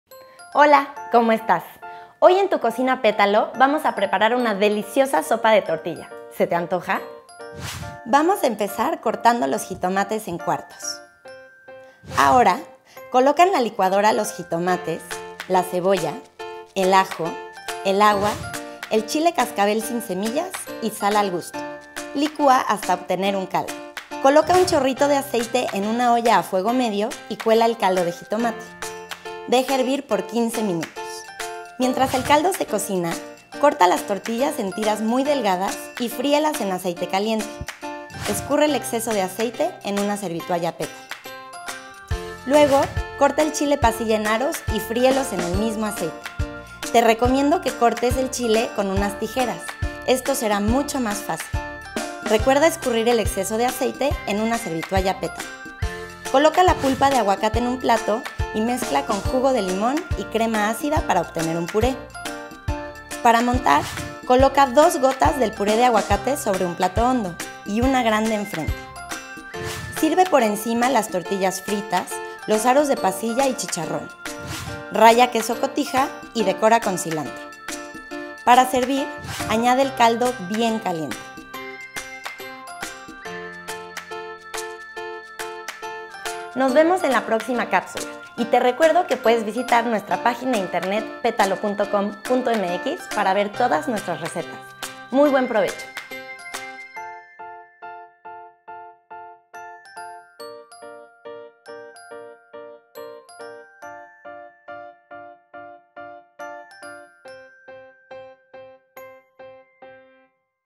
Sopa de Tortilla - Tu Cocina Pétalo®
Bienvenida a Tu Cocina Pétalo®. Hoy traemos para ti una receta para preparar una deliciosa Sopa de Tortilla. Disfrútala con tu familia!

Ingredientes:
 - 4 jitomates
 - 1/2 cebolla
 - 1 diente de ajo
 - 1.5 lts de agua
 - 1 chile cascabel
 - Aceite para freír
 - Tortillas
 - 1 chile pasilla
 - Brotes de cilantro
 - Aguacate
 - Orégano
 - Queso cotija
 - Crema ácida
 - Cicharrón de cerdo